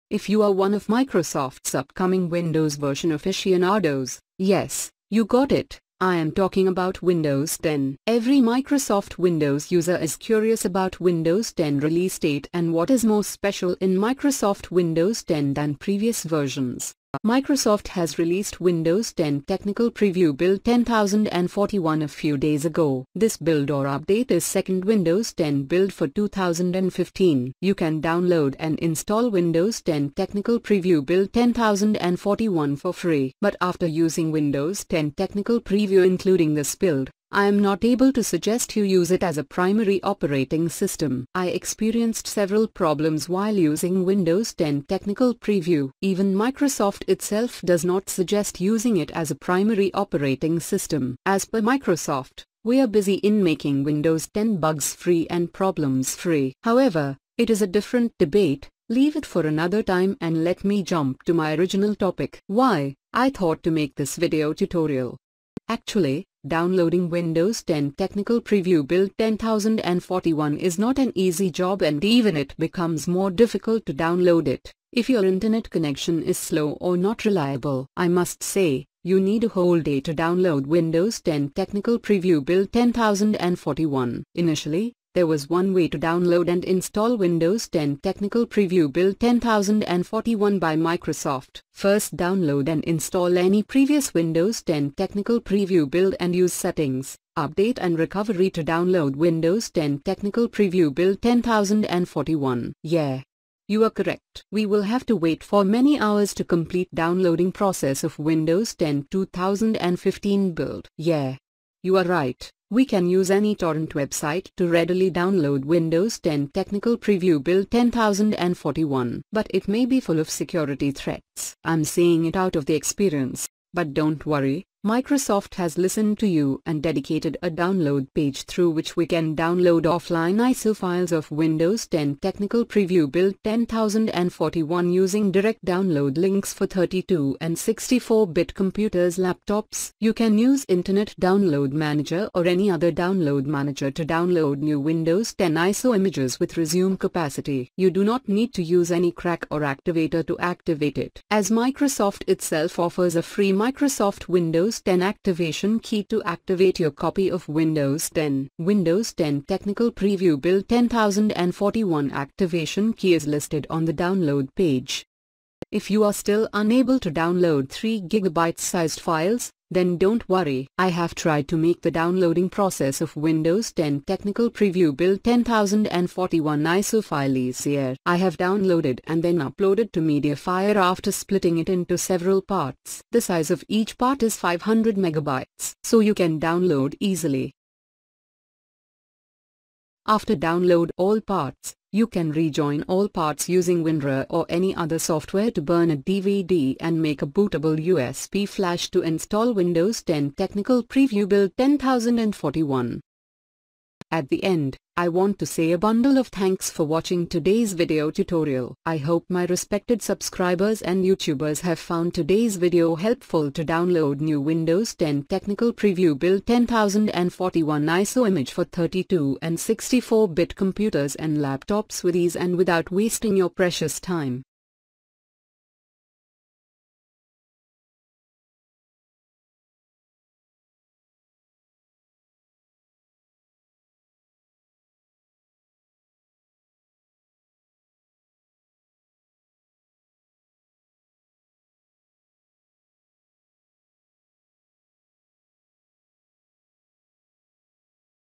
How To Download Windows 10 Technical Preview build 10041 ISO Offline 32/64 Bit
How to download Windows 10 Technical Preview build 10041 for free and with ease.

If you are facing problems in downloading Windows 10 Technical Preview build 10041 (Windows 10 2015), then this video can help you download Windows 10 Technical Preview build 10041 ISO image file with direct download links for 32 and 64 bit PC and laptops. After downloading it, You burn a DVD and make a bootable USB flash to install Windows 10 Technical Preview build 10041. You can also activate using given activation key without crack or any activator.

If you are one of Microsoft's upcoming Windows version aficionados, Yes, You got it, I am talking about Windows 10. Every Microsoft Windows user is curious about Windows 10 release date and What is more special in Microsoft Windows 10 than previous versions.

Microsoft has released Windows 10 Technical Preview build 10041 a few days ago. This build or update is second Windows 10 build for 2015. You can download and install Windows 10 Technical Preview build 10041 for free. But After using Windows 10 Technical Preview including this build, I am not able to suggest you use it as a primary operating system. I experienced several problems while using Windows 10 Technical Preview. Even Microsoft itself does not suggest using it as a primary operating system. As per Microsoft, We are busy in making Windows 10 bugs-free and problems-free. However, It is a different debate, leave it for another time and let me jump to my original topic. Why, I thought to make this video tutorial?

Actually, Downloading Windows 10 Technical Preview build 10041 is not an easy job and Even it becomes more difficult to download it, If your internet connection is slow or not reliable. I must say, You need a whole day to download Windows 10 Technical Preview build 10041. Initially, There was one way to download and install Windows 10 Technical Preview build 10041 by Microsoft. First Download and install any previous Windows 10 Technical Preview build and use Settings, Update and recovery to download Windows 10 Technical Preview build 10041. Yeah! you are correct. We will have to wait for many hours to complete downloading process of Windows 10 2015 build. Yeah! you are right, We can use any torrent website to readily download Windows 10 Technical Preview build 10041. But It may be full of security threats. I'm saying It out of the experience.

But Don't worry, Microsoft has listened to you and dedicated a download page through which We can download offline ISO files of Windows 10 Technical Preview build 10041 using direct download links for 32 and 64 bit computers laptops. You can use Internet Download Manager or any other Download Manager to download new Windows 10 ISO images with resume capacity. You do not need to use any crack or activator to activate it. As Microsoft itself offers a free Microsoft Windows 10 activation key to activate your copy of Windows 10. Windows 10 Technical Preview build 10041 activation key is listed on the download page.

If you are still unable to download 3 GB sized files, Then don't worry. I have tried to make the downloading process of Windows 10 Technical Preview build 10041 ISO file easier. I have downloaded and then uploaded to Mediafire after splitting it into several parts. The size of each part is 500 MB. So You can download easily.

After download all parts, You can rejoin all parts using Winrar or any other software to burn a DVD and make a bootable USB flash to install Windows 10 Technical Preview build 10041.

I hope my respected subscribers and YouTubers have found today's video helpful to download new Windows 10 Technical Preview build 10041 ISO image for 32 and 64 bit computers and laptops with ease and without wasting your precious time.